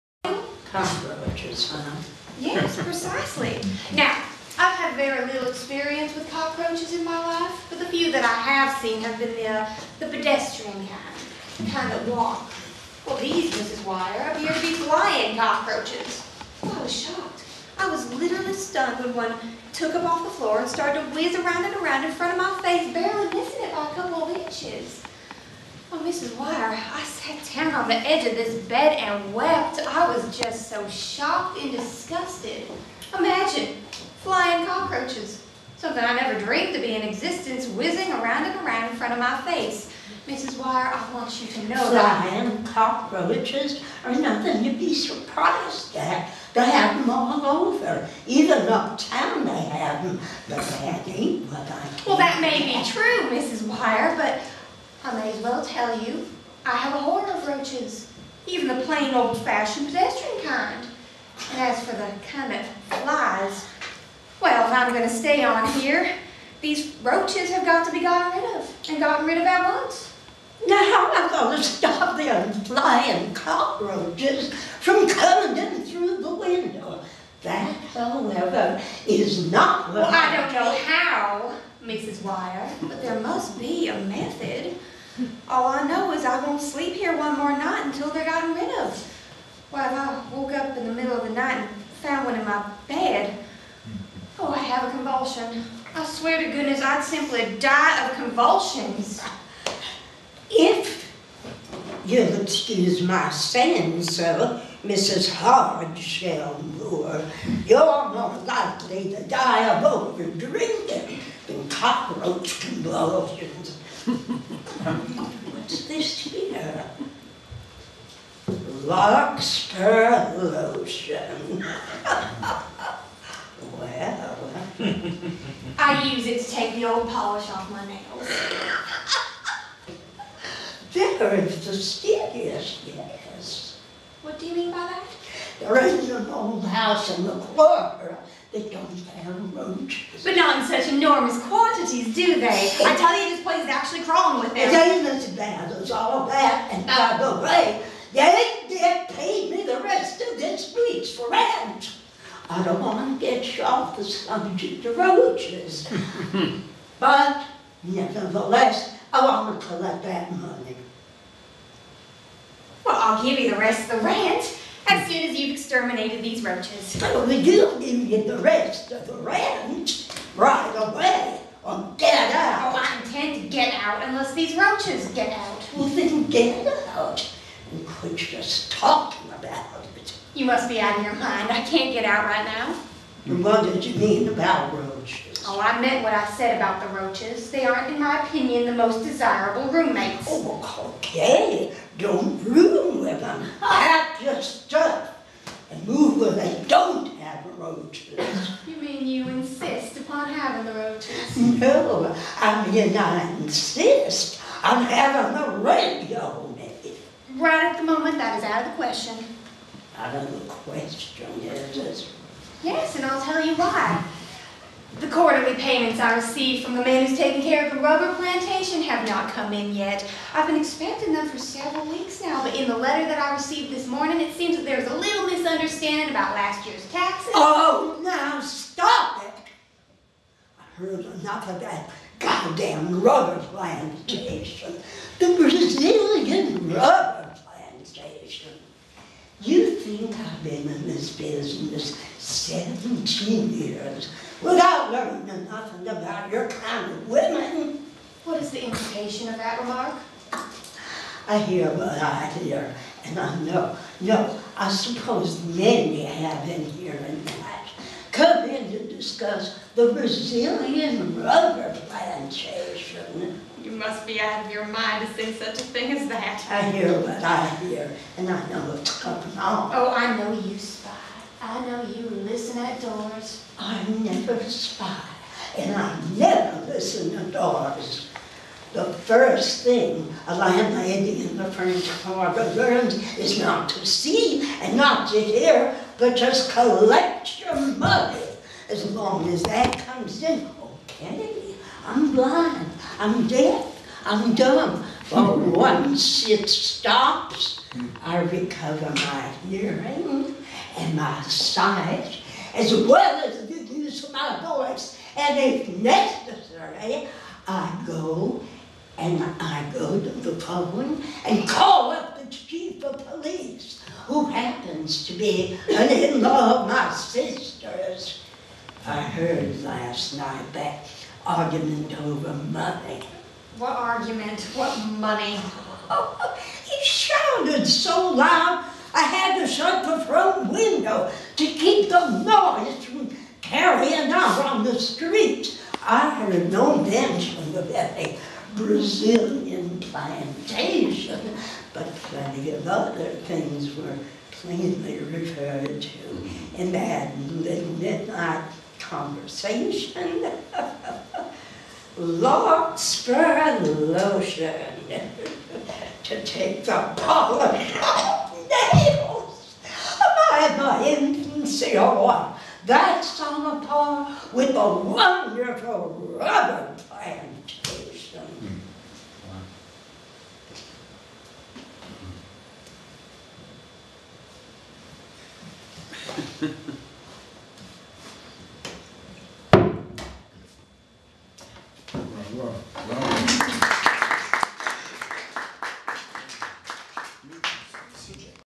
Scene: Lady of Larkspur Lotion, Tennessee Williams
Tennessee Williams Birthday Celebration at the Simon Studio, April 1, 2017: Lady of Larkspur Lotion with Chelsea Clark and C.J. Gelfand